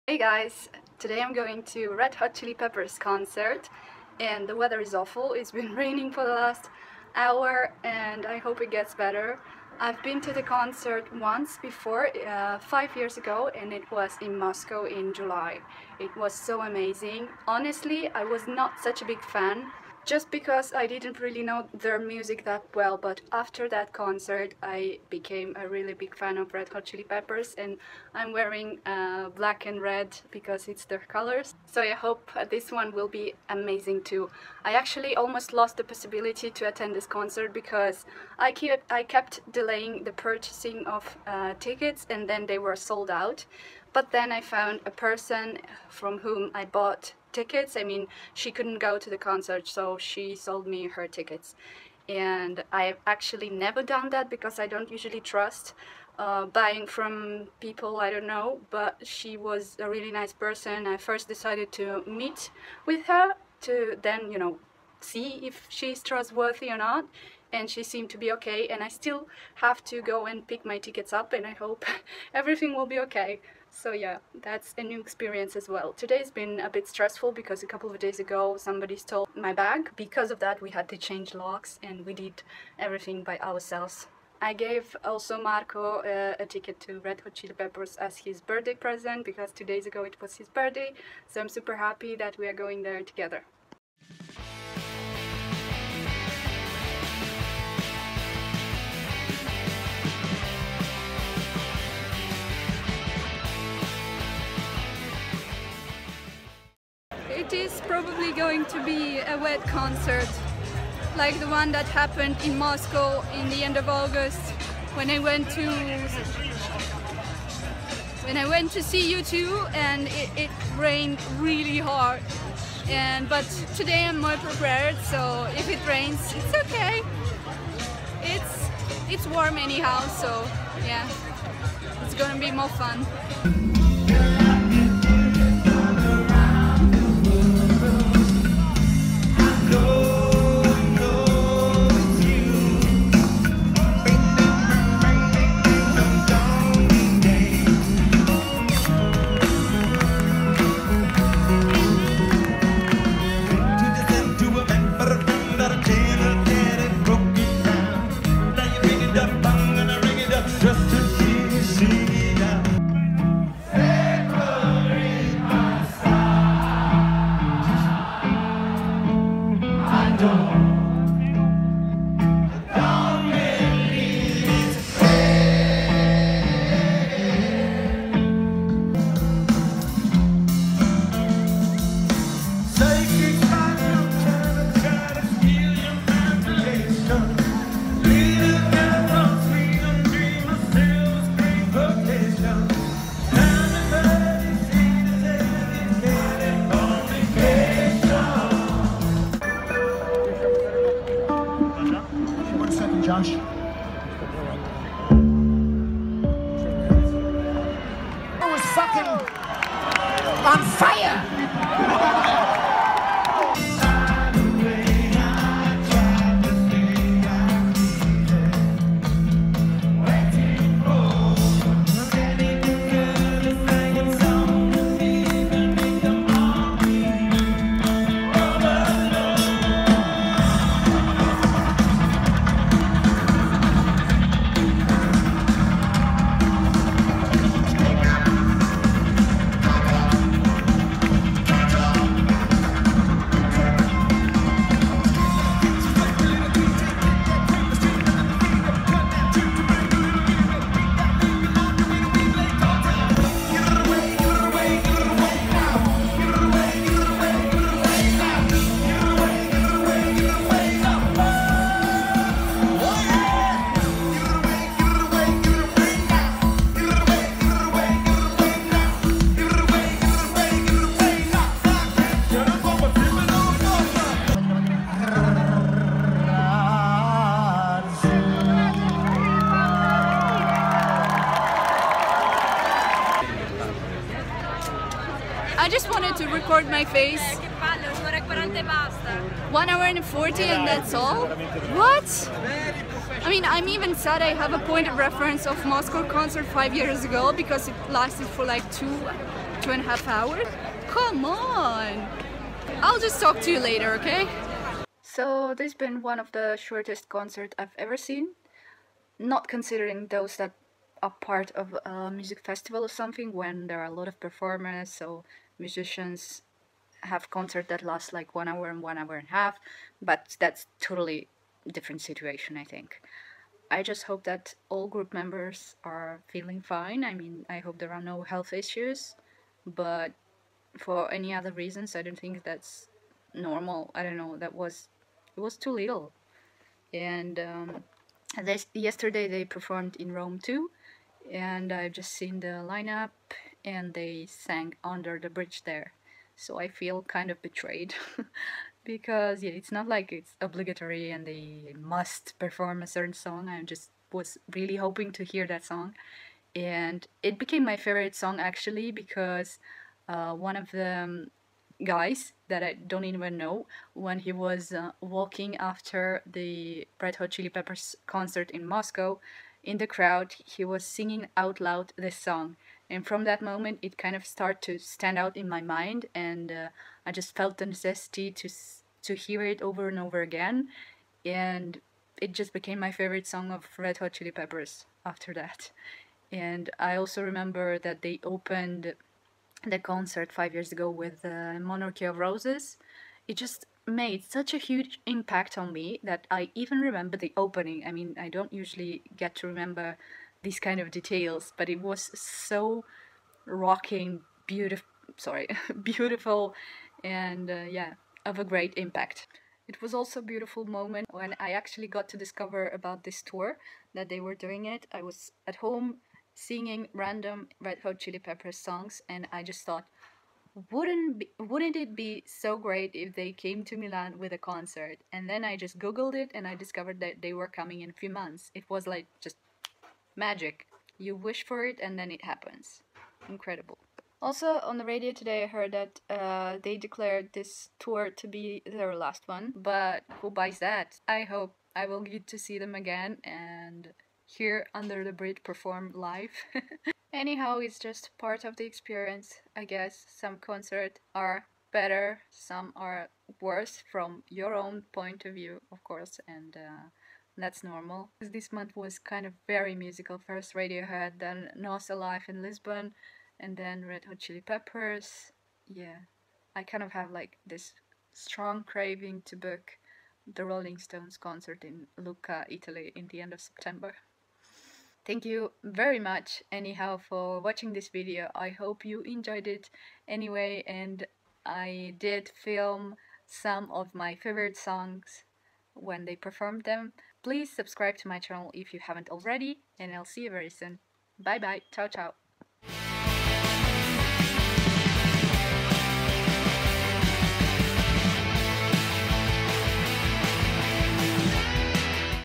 SHORTEST CONCERT EVER?! | Red Hot Chili Peppers Milan Concert Vlog | MUSIC | Dilya London
Red Hot Chili Peppers in Milan live concert, 21 of July, 2017.

This is the second RHCP concert for me, even though I found it rather short, it was still enjoyable.

What was the shortest concert you have ever attended? ;)